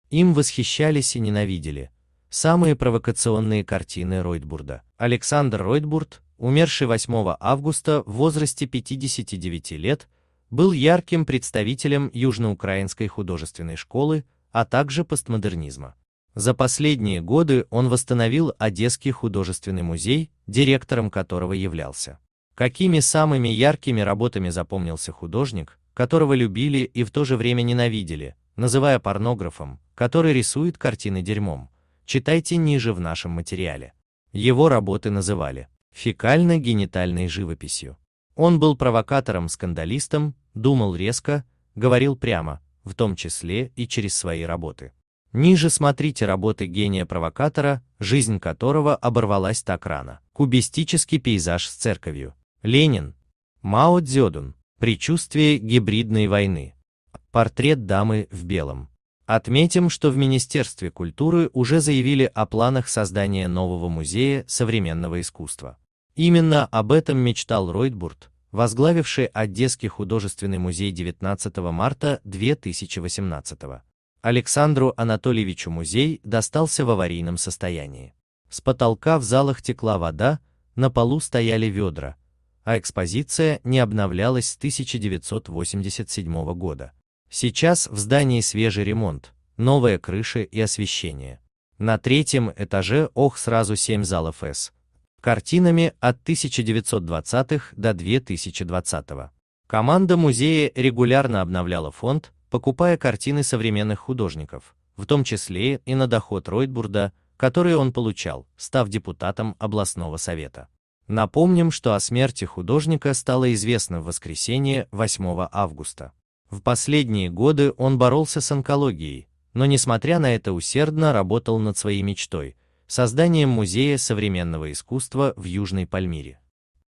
Им восхищались и ненавидели: самые провокационные картины Ройтбурда.
художник АлександрРойтбурд Александр Ройтбурд, умерший 8 августа в возрасте 59 лет, был ярким представителем Южноукраинской художественной школы, а также постмодернизма. За последние годы он восстановил Одесский художественный музей, директором которого являлся. Какими самыми яркими работами запомнился художник, которого любили и в то же время ненавидели, называя "порнографом, который рисует картины дерьмом" - читайте ниже в нашем материале. Его работы называли "фекально-генитальной живописью". Он был провокатором, скандал